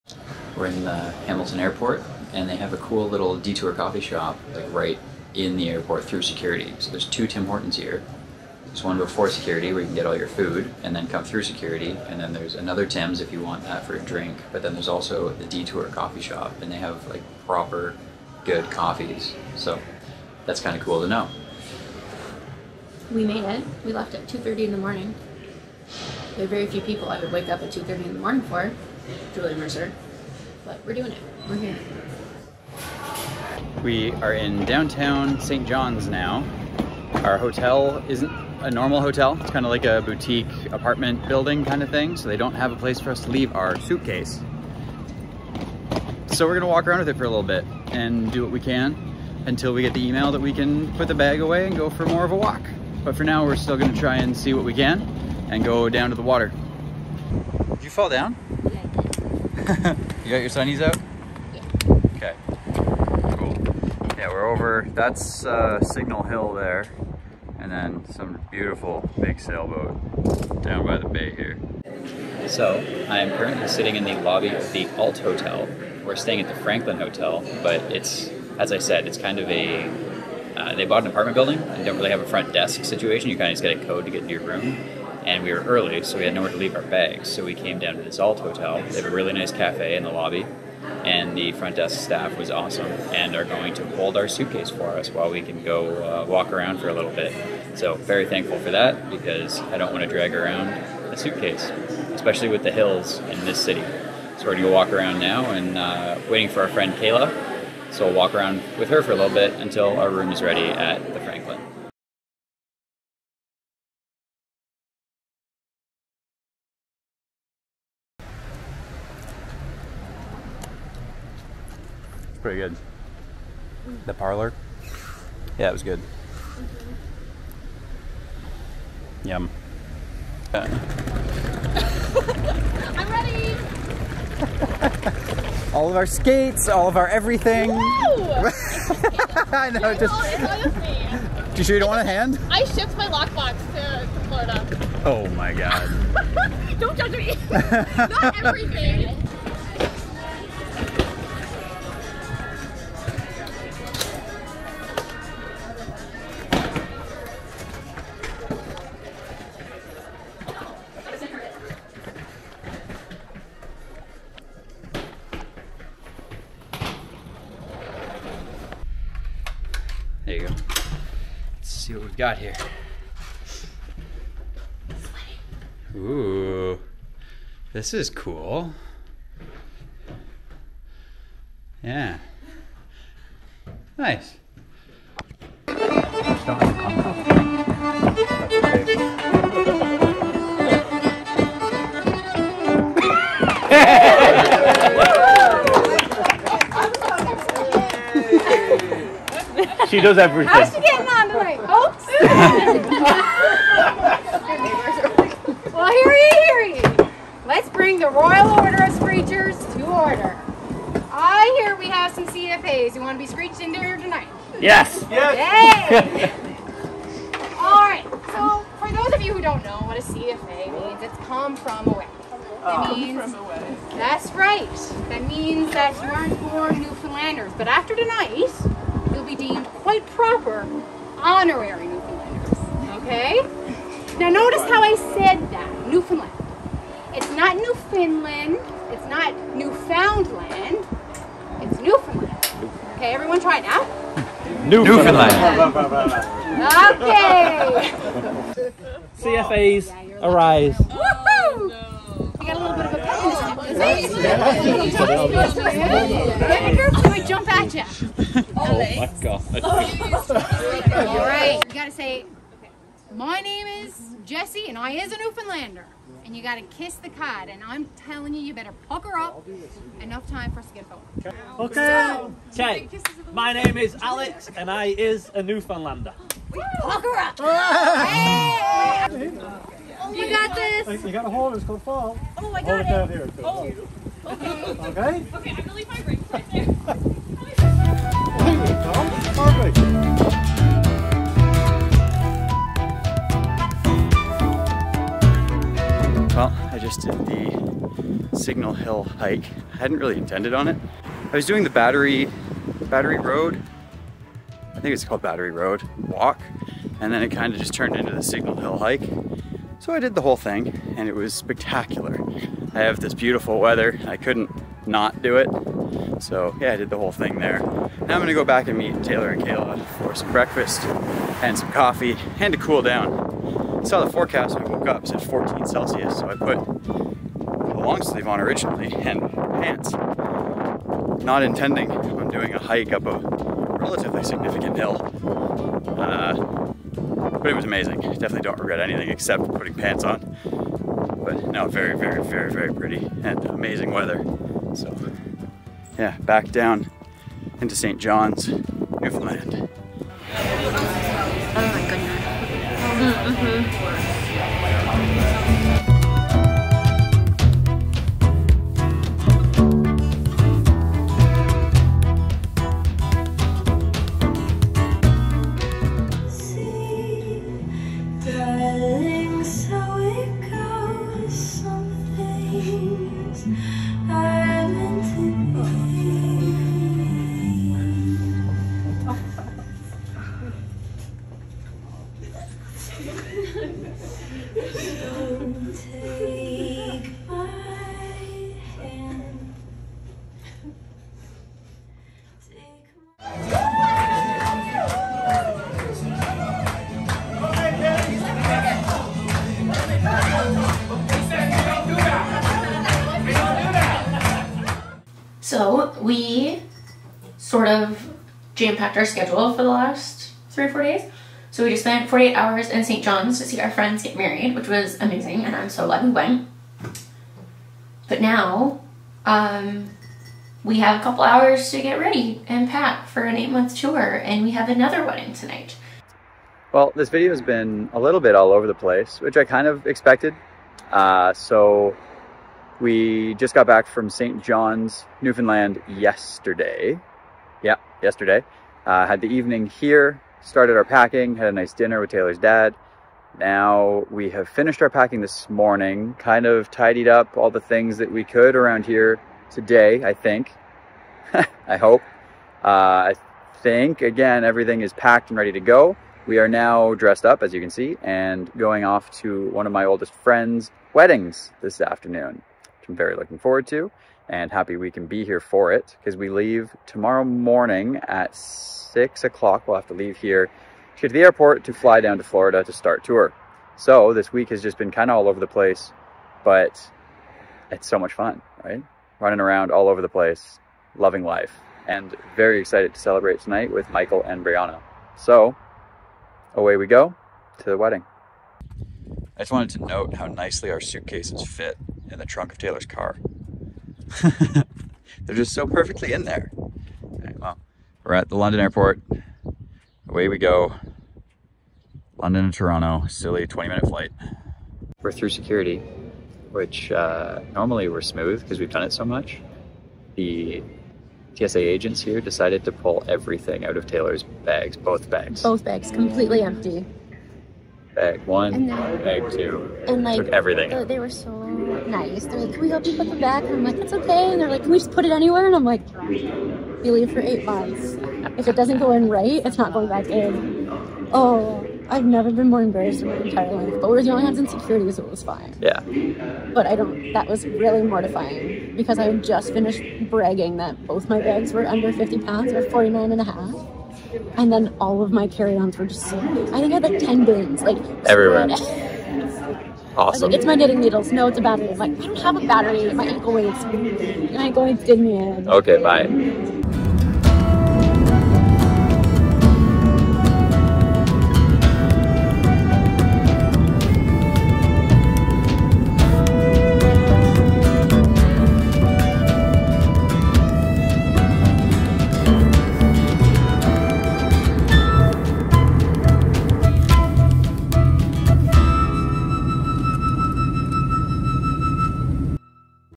St John's NL wedding and travel to tour #12 | VLOG
We have a whirlwind week, London Ontario to St. John's NL and back, then we head to Florida to begin a 9 month tour!